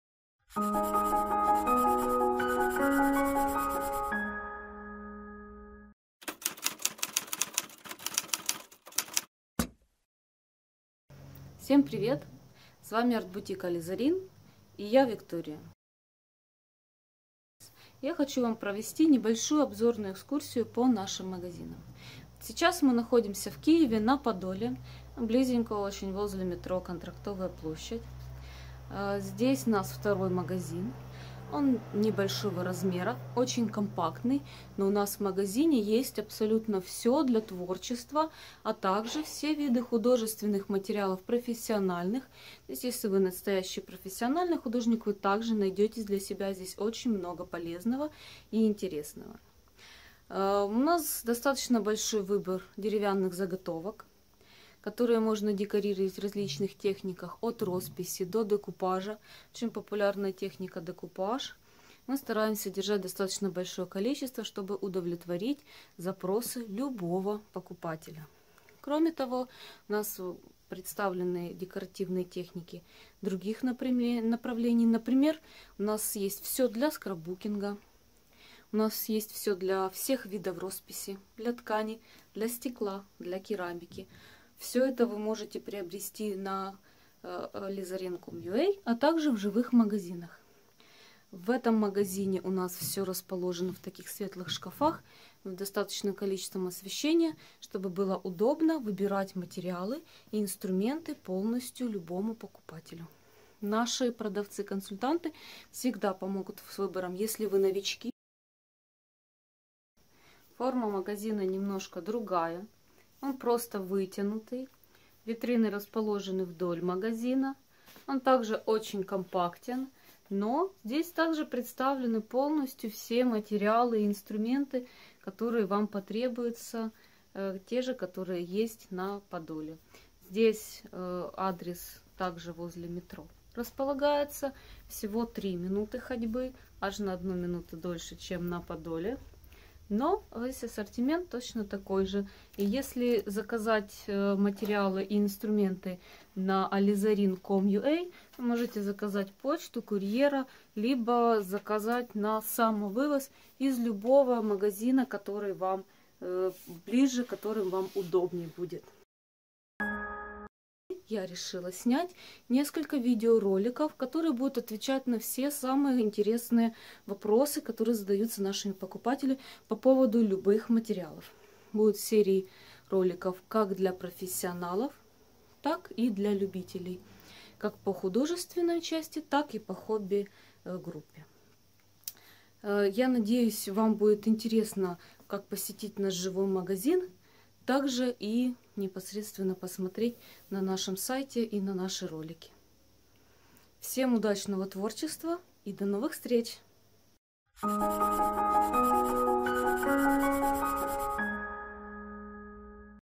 Знакомство с сетью арт-бутиков "Ализарин"
Сеть арт-бутиков "Ализарин" предлагает широкий выбор материалов для творчества и хобби по низким ценам проверненого качества.

Для художников низкие цены на лучшие масляные краски, кисти и холсты или акварельные краски и бумагу высокого качества, а так же широкий выбор мольбертов.

Для творческих людей прекрасный выбор материалов для росписи по стеклу и керамике или же рисунка по ткани.

Для любителей декупажа и декорирования огромный выбор заготовок из дерева, декупажных салфеток и карт, клея для декупажа, трафареты и различных материалов для декорирования различных поверхностей.

Фанаты скрапа смогут подобрать все для скрапбукинга от альбомов, бумаги, чипбордов и штампов до фишек и стикеров.

Адреса арт-бутиков "Ализарин":

Художественные материалы на Подоле
Украина, г. Киев, ул. Нижний Вал, 33
Станция метро Контрактовая площадь

Художественные материалы в Центре Киева
Украина, г. Киев, ул. Большая Васильковская, 30В
Станция метро площадь Льва Толстого